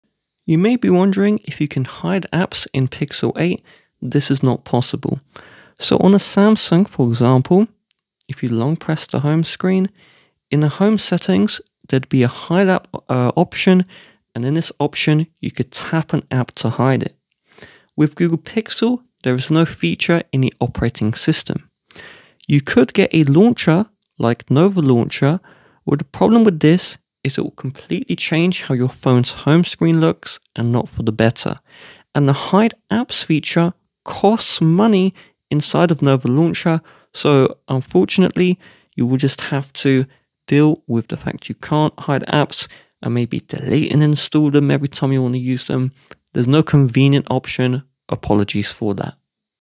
Can You Hide Apps in Google Pixel 8? no
In this video, we’ll address whether you can hide apps on the Google Pixel 8. We’ll explain the current options and limitations regarding app visibility on this device. Learn about alternative methods for managing app access and keeping your home screen organized. Follow along to understand how to handle app visibility on your Google Pixel 8.